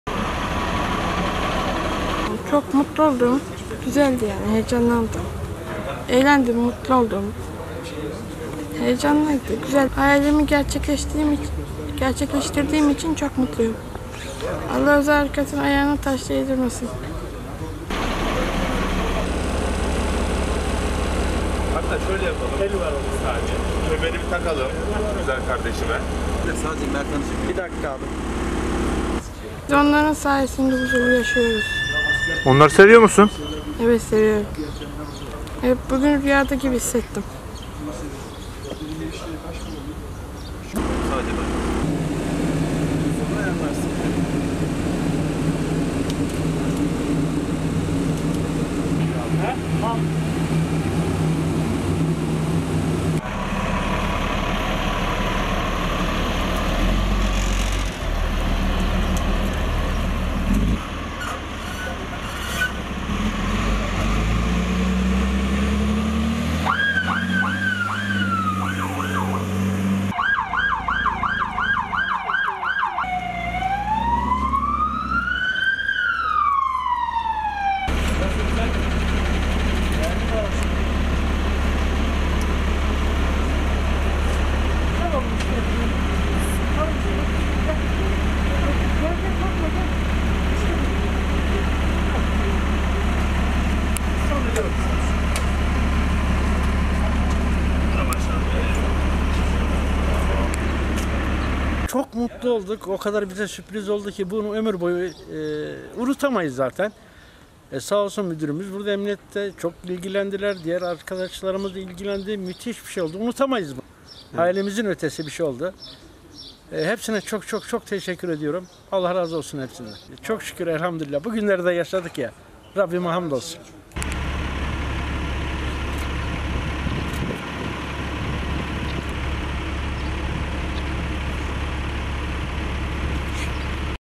14 yaşındaki kas erime hastası Merthan'ın özel harekat polisi olma hayalini gerçekleştirdiler
◥ Denizli Emniyet Müdürlüğü, 14 yaşındaki kas erime hastası Merthan Marifet'in özel harekat polisi olma hayalini gerçekleştirdi.

◥ Merkezefendi ilçesinde yaşayan baba Muhittin Marifet, İl Emniyet Müdürlüğü yetkililerine çocuğunun özel harekat polisi olma hayali kurduğunu iletti.
Bunun üzerine Özel Harekat Şube Müdürlüğüne davet edilen Merthan Marifet, burada kendisine hediye edilen özel harekat şapkasını taktı, tişörtünü giydi.

◥ Marifet, daha sonra bahçede bulunan yerli yapım taktik tekerlekli zırhlı araç Ejder Yalçın'ın tanıtımından sonra araca bindirildi.

◥ Kendisine refakat eden özel harekat polisleriyle zırhlı araçla kurum çevresinde tur atan Marifet siren çalarak mutluluğunu katladı.
Marifet, AA muhabirine çocukluğundan beri emniyet mensuplarını çok sevdiğini ve polis olmak istediğini söyledi.

◥ Hastalığı nedeniyle yürüme yetisini kaybettiğini belirten Merthan Marifet, "Çok mutlu oldum. Heyecan vericiydi. Araç çok güzeldi ve eğlendim. Polis olmak hayalimdi. Hayalimi gerçekleştirdiğim için çok mutluyum. Allah'ım özel harekatın ayağına taş değdirmesin inşallah. Biz onların sayesinde huzurlu yaşıyoruz. Onları çok seviyorum. Bugün kendimi rüyada gibi hissettim." diye konuştu.

◥ Muhittin Marifet de evladının 3 yaşındayken hastalığa yakalandığını ve zamanla yürüme yeteneğini kaybederek tekerlekli sandalye kullanmak zorunda kaldığını anlattı.

◥ Oğlunun hayalini gerçekleştiren polis ekiplerine teşekkür eden Marifet şöyle konuştu:

◥ "Oğlumun tek hayali özel harekat polisi olmaktı. Bugünleri de yaşadık. Rabb'ime hamdolsun. Çok mutlu olduk. Bu anları ömür boyu unutmayacağız. Sağ olsun müdürüm çok ilgilendiler. Müthiş bir gün oldu. Bugünü yaşadık ya, hayal ötesi bir gündü. Allah razı olsun herkesten."